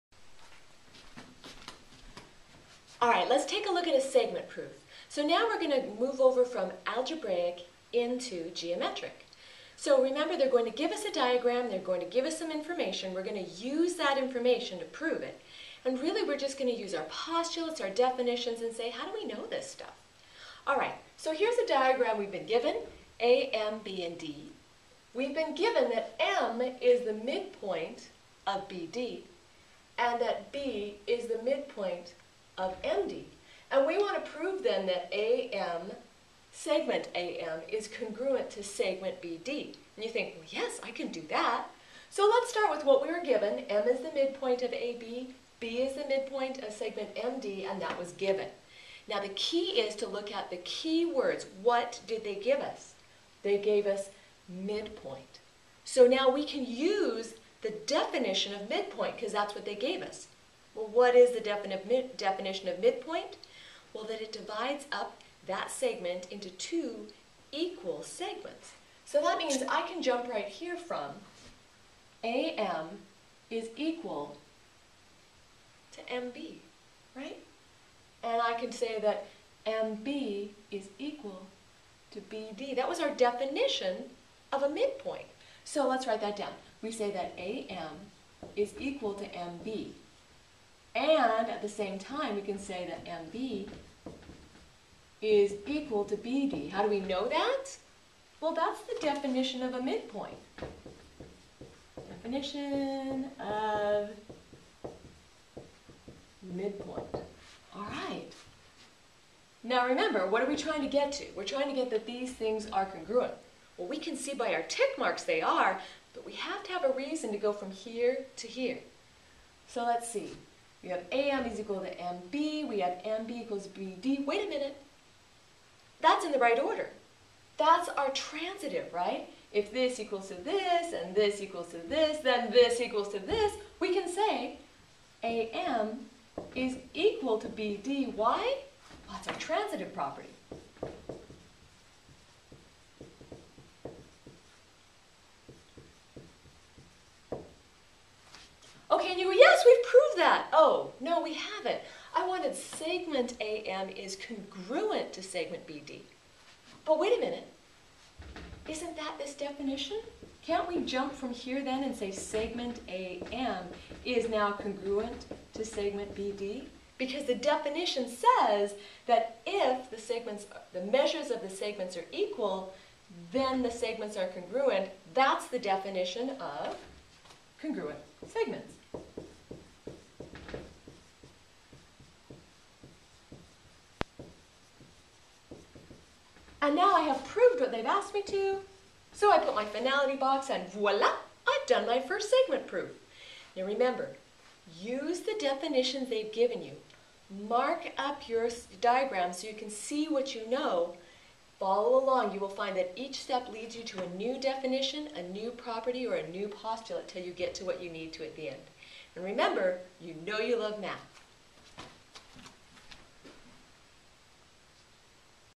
Geometry Segment Proof
Example of a segment proof. Use the given diagram and definitions, postulates and theorems on segments to prove the statement provided.